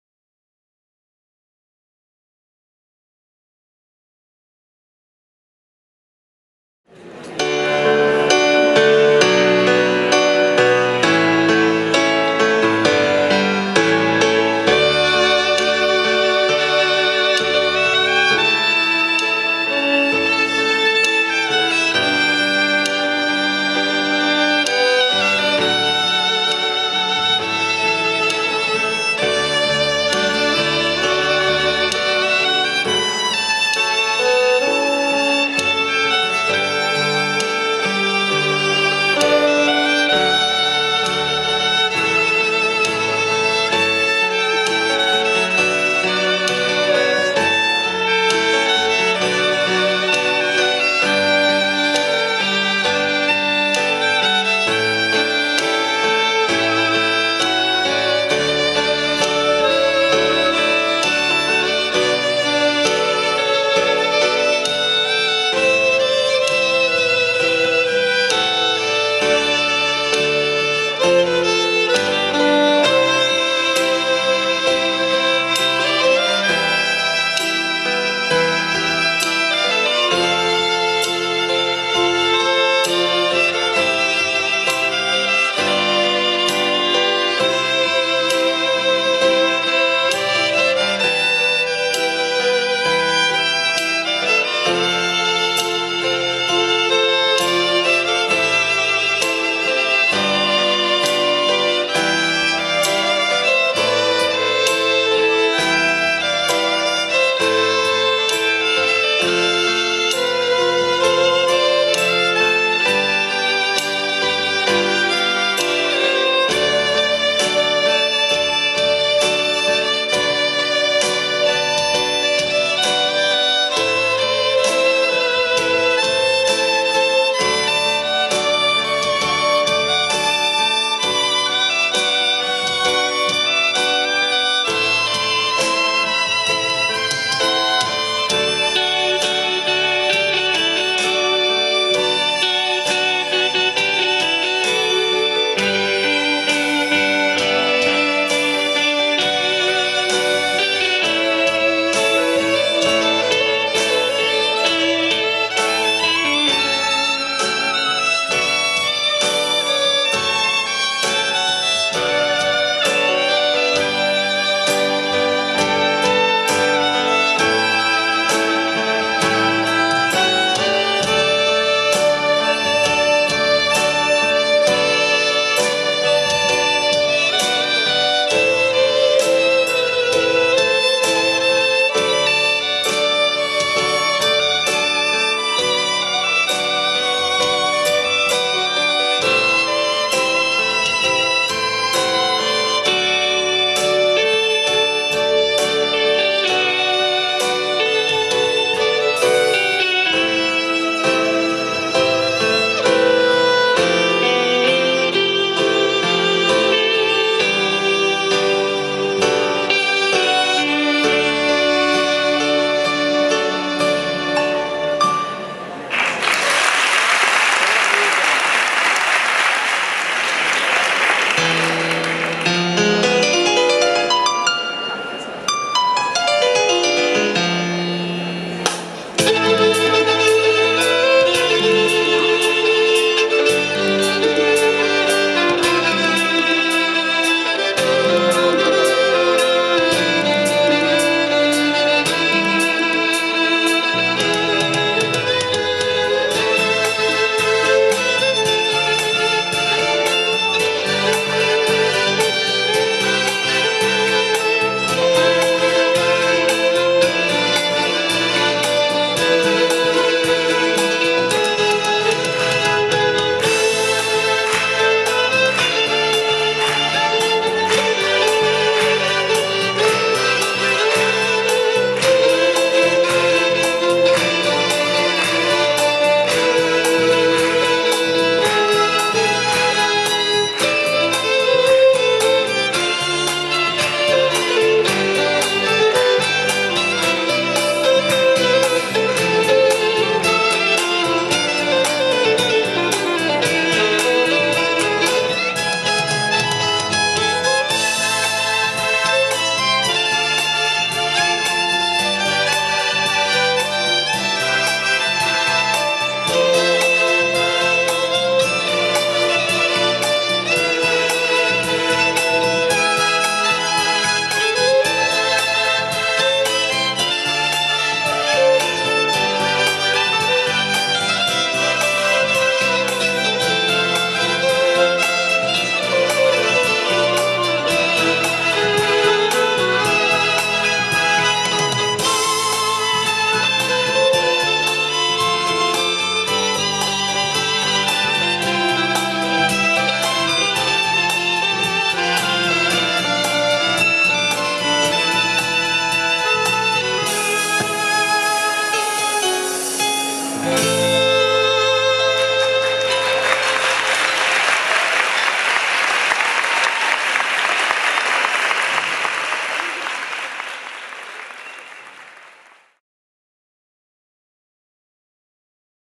ExpressArt Dúo
Actuación en la Graduación en el "IES Bioclimático",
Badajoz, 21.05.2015